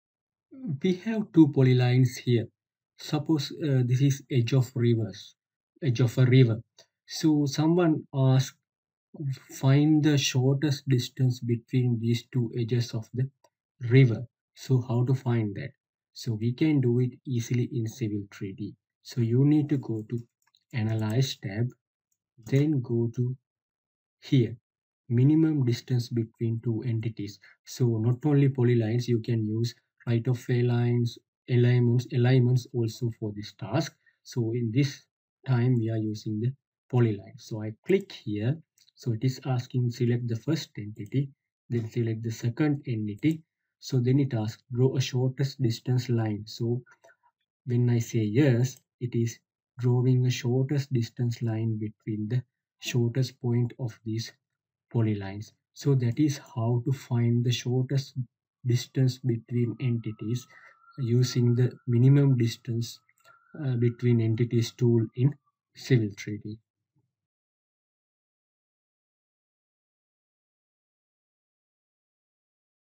Find the Shortest Distance Between 2 entities in Civil 3D
we have two poly lines here suppose this is Edge of Rivers Edge of a river so someone ask find the shortest distance between these two edges of the river so how to find that so we can do it easily in civil 3D so you need to go to analyze tab then go to here minimum distance between two entities so not only poly lines you can use right of a lines alignments alignments also for this task so in this time we are using the poly line so I click here so it is asking select the first entity then select the second entity so then it asks draw a shortest distance line so when I say yes it is drawing a shortest distance line between the shortest point of these poly lines so that is how to find the shortest distance between and entities using the minimum distance uh between entities tool in civil 3D.